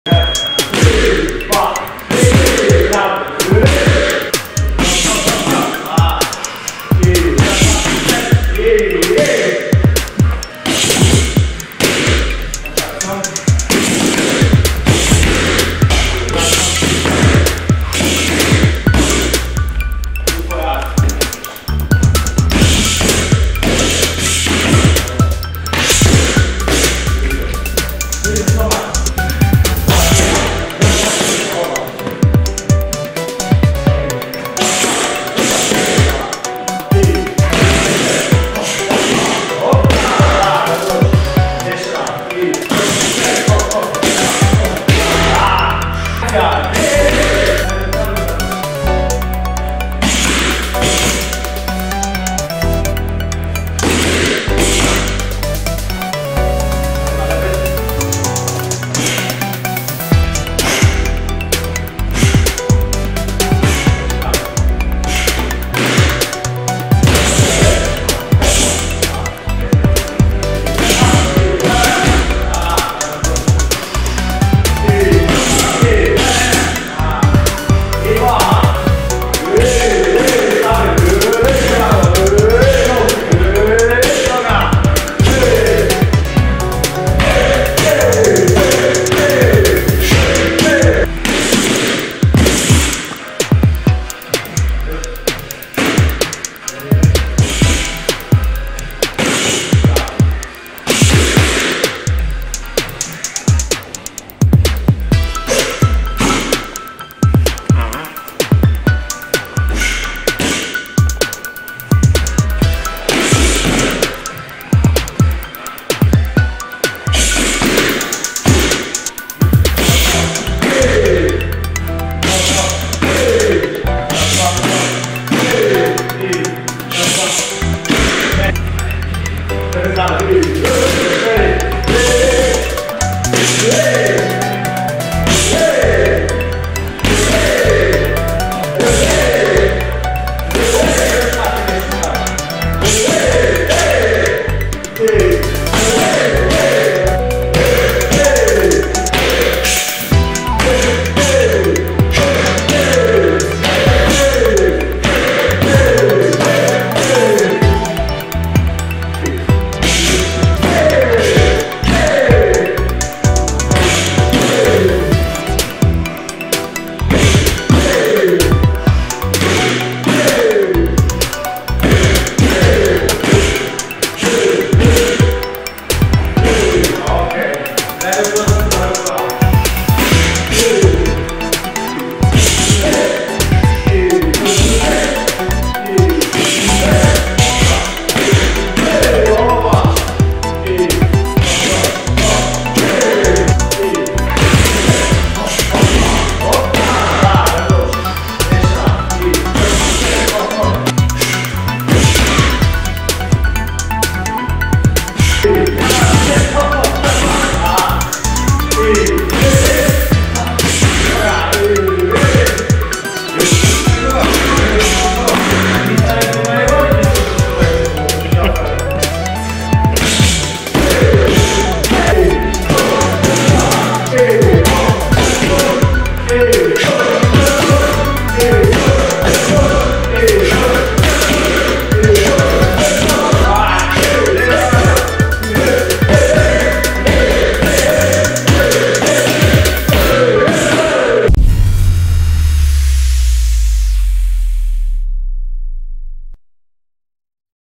Trener BJJ Artur Gwóźdź na treningu kickboxerskim
- Portal o wszystkich sportach walki przedstawia fragmenty treningu trenera BJJ.